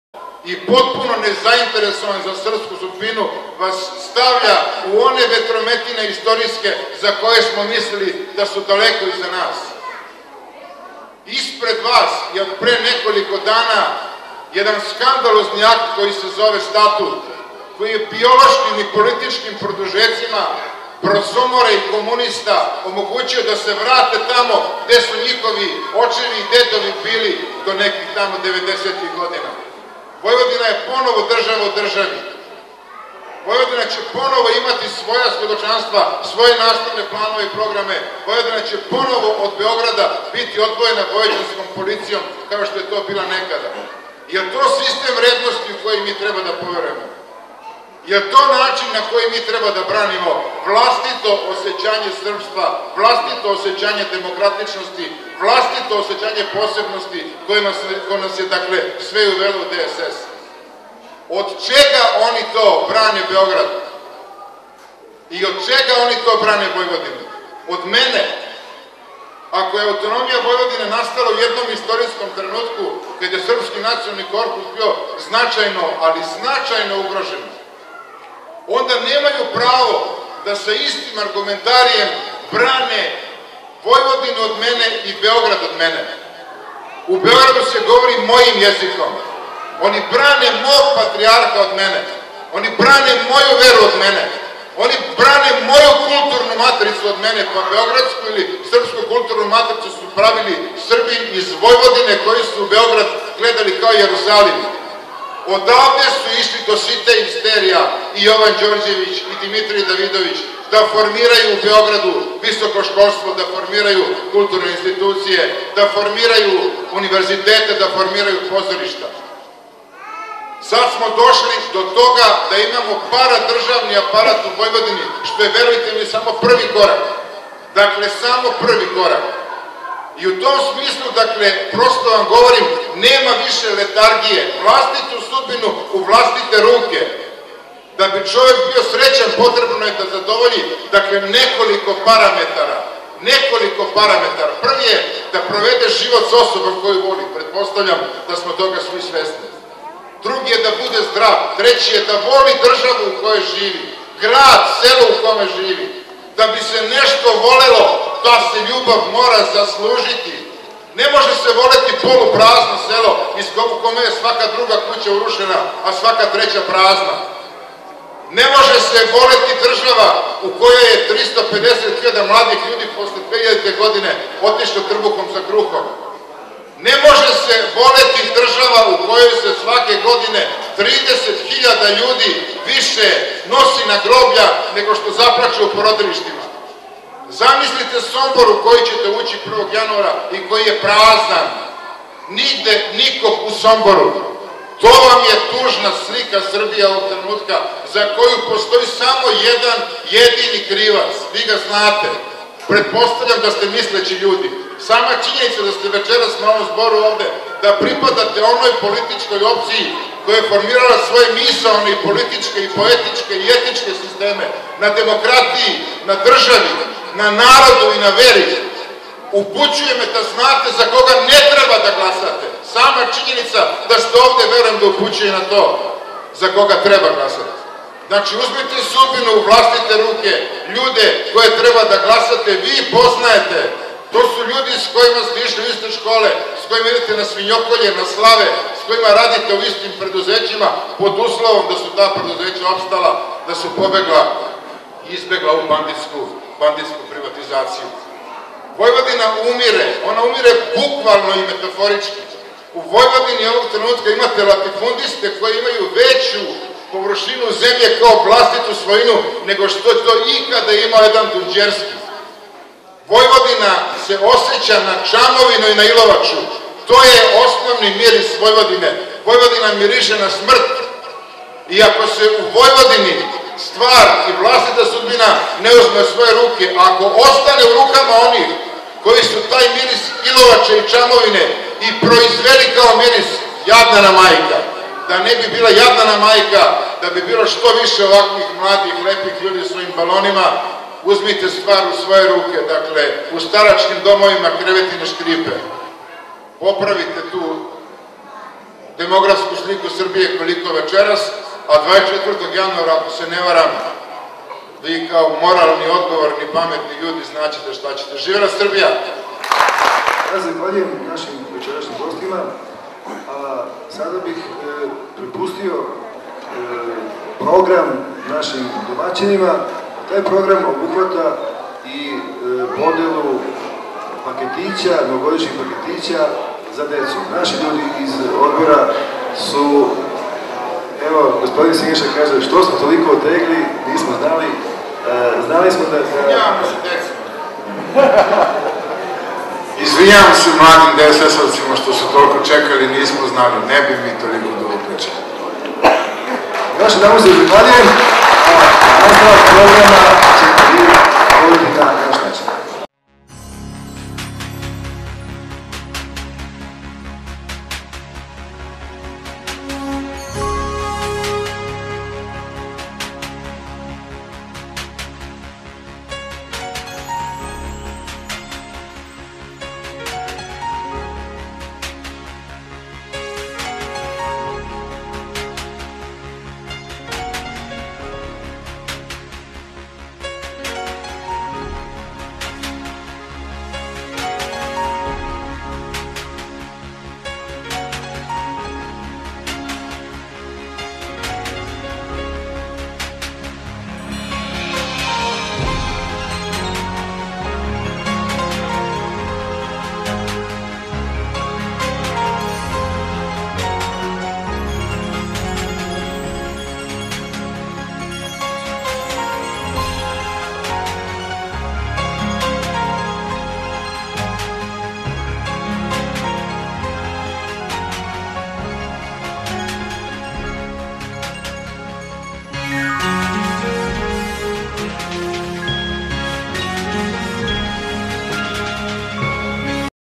Tribina u Ratkovu - 3. Deo
Snimak sa tribine u Ratkovu. Govore: Vladimir Košutić, Stojan Đorđević, Željko Tomić, Borko Ilić, Siniša Kovačević. Na kraju tribine deci su podeljeni paketići u organizaciji MO Ratkovo.